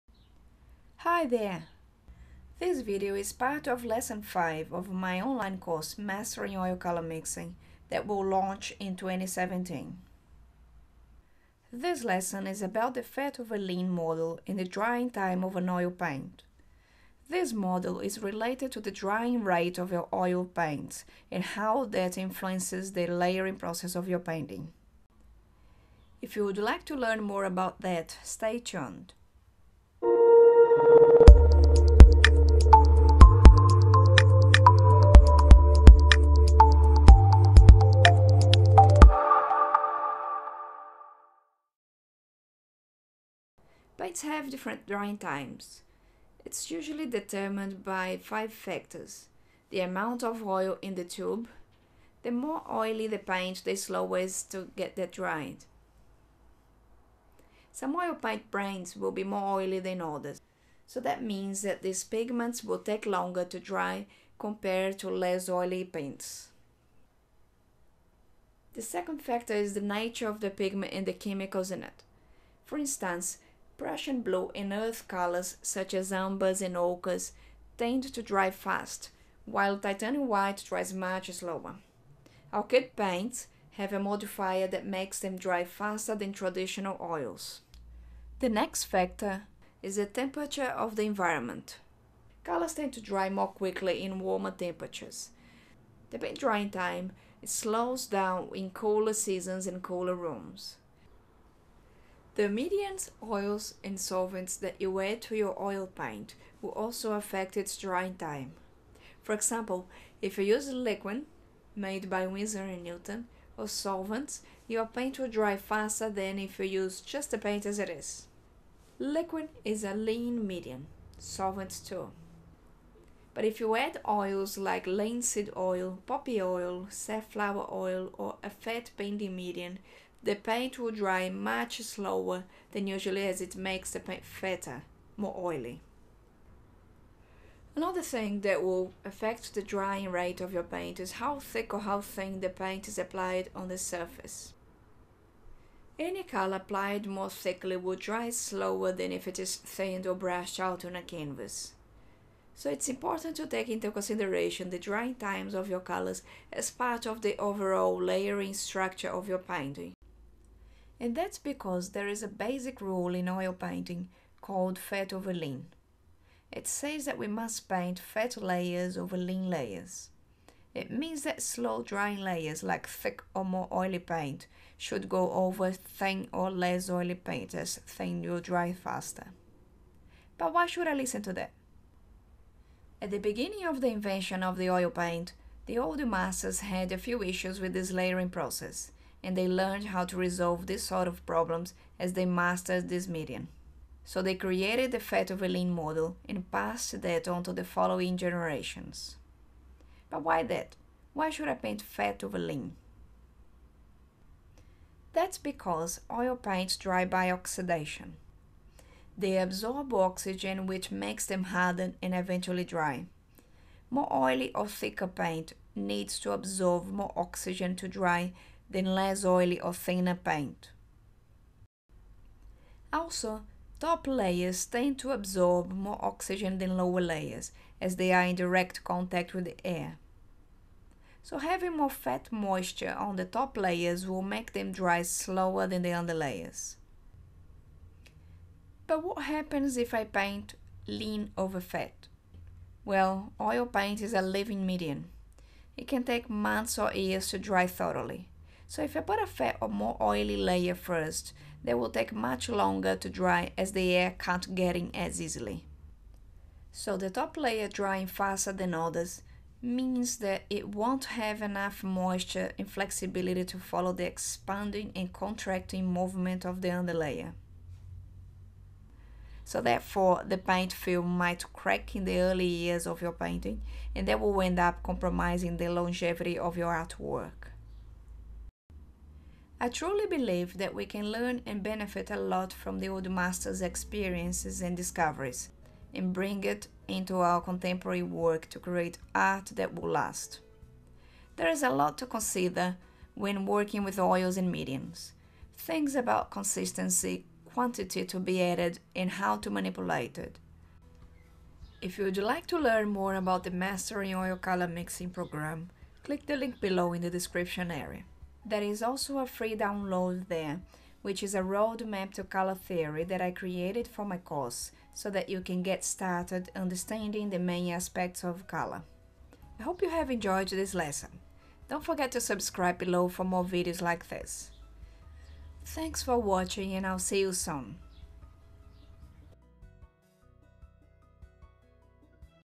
FAT OVER LEAN RULE in Oil Painting & Drying Time
This is a tutorial about the Fat over Lean Rule in Oil Painting and the Drying Time of oil paints.

In this video, you will learn what the Fat over Lean rule means, how the drying time of oil paint applies to this rule, why it's important to follow this model. It also explains about the layering structure of an oil painting.

LIVIA DIAS
Artist and Tutor